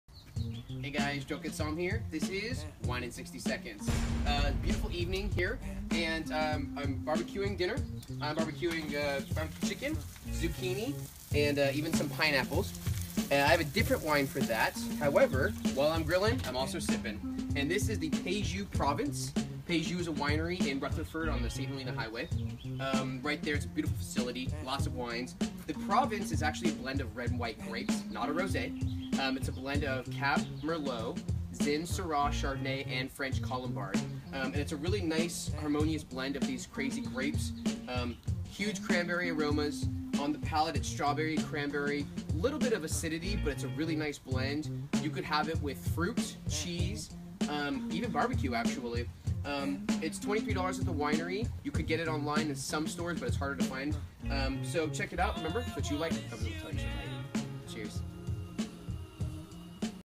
Wine in 60 Seconds Episode 101 - Good Anytime Wine
Peju is a beautiful winery in Rutherford, CA right in the St. Helena Highway in the center of Napa Valley. They have a ton of wines they make and many are great but the Provence is a unique blend of red and white wines not commonly seen, especially with one of the grapes being extremely uncommon itself. It’s a blend of Cabernet Sauvignon, merlot, Zinfandel, syrah, chardonnay, and French colombard and has strong cranberry on the nose as soon as you outnitnupntonyour nose. You even get hints of cranberry along with the strawberry on the palate. If you visit the winery it’s only $23 and definitely something you should try because it’ll pair well with cheeses, fruit, or even barbecue. I do want to point out that even though it’s a blend of red and white wine, it is NOT a rosé.